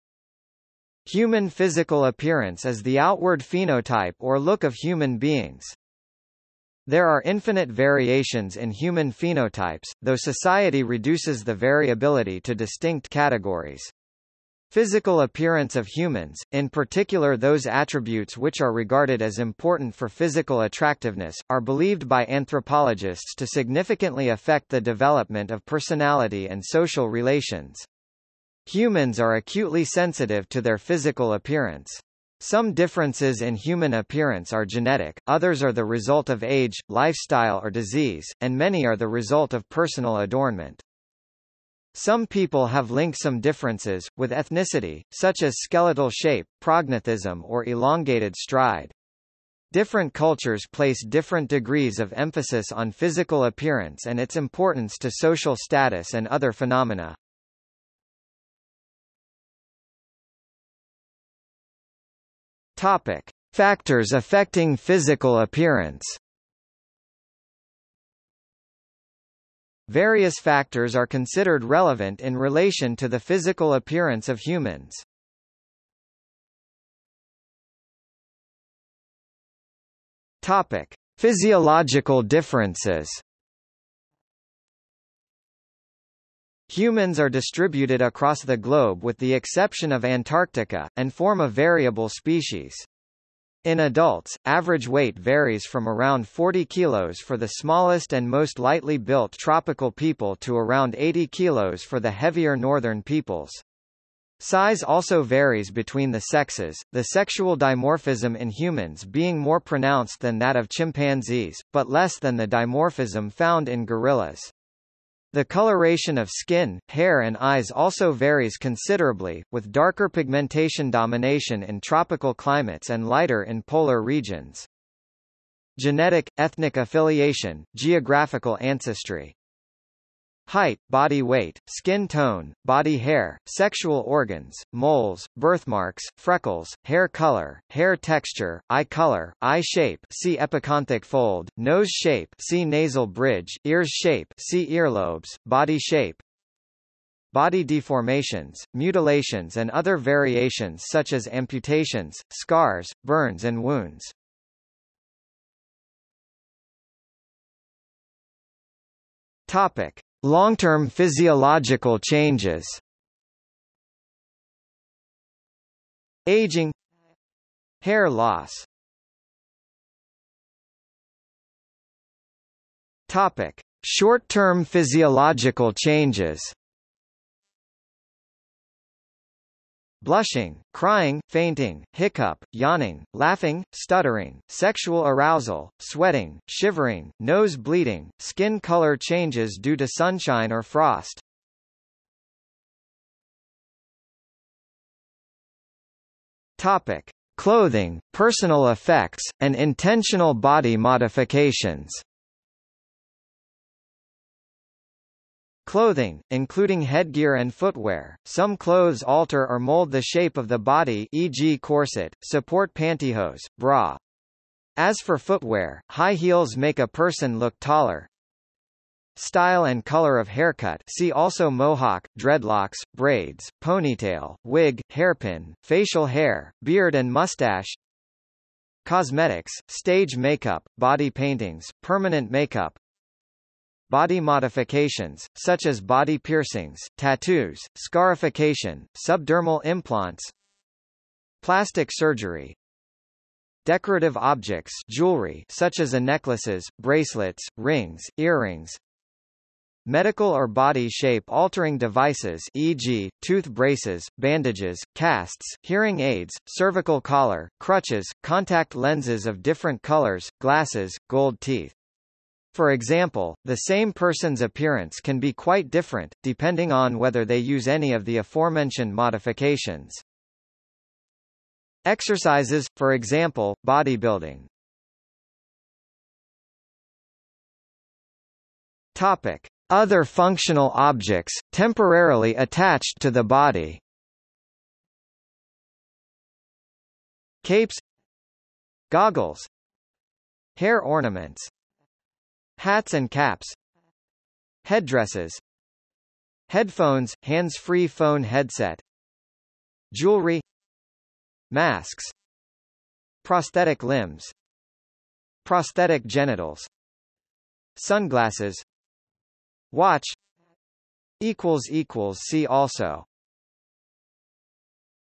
Human physical appearance | Wikipedia audio article
00:00:59 1 Factors affecting physical appearance
00:01:17 1.1 Physiological differences
00:02:36 1.2 Long-term physiological changes
00:02:49 1.3 Short-term physiological changes
00:03:13 1.4 spanClothing, personal effects, and intentional body modifications
00:04:35 1.5 Other functional objects, temporarily attached to the body

- Socrates

SUMMARY
Human physical appearance is the outward phenotype or look of human beings.

There are infinite variations in human phenotypes, though society reduces the variability to distinct categories. Physical appearance of humans, in particular those attributes which are regarded as important for physical attractiveness, are believed by anthropologists to significantly affect the development of personality and social relations. Humans are acutely sensitive to their physical appearance.  Some differences in human appearance are genetic, others are the result of age, lifestyle or disease, and many are the result of personal adornment.
Some people have linked some differences, with ethnicity, such as skeletal shape, prognathism or elongated stride. Different cultures place different degrees of emphasis on physical appearance and its importance to social status and other phenomena.